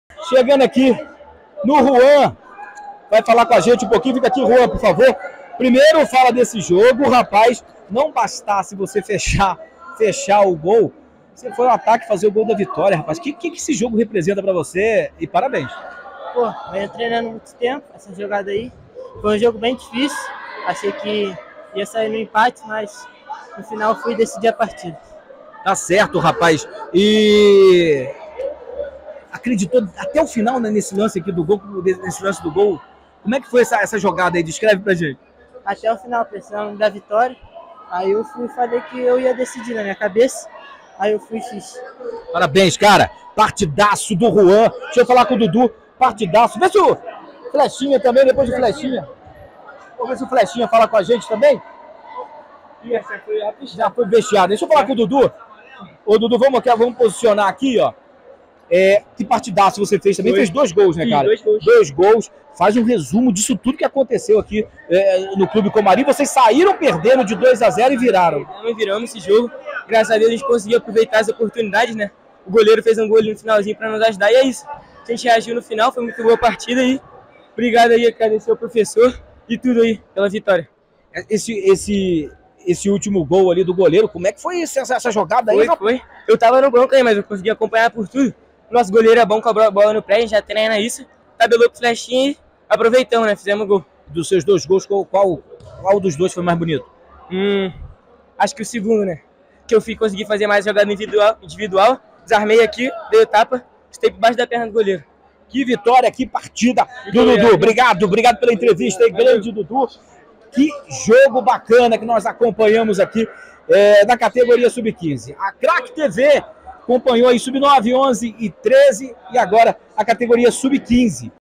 ➡️🎤ENTREVISTAS: 5ª rodada do Estadual da Rio Futsal sub 15 - Comary 4 X 3 Ceros…Assista!
Craque TV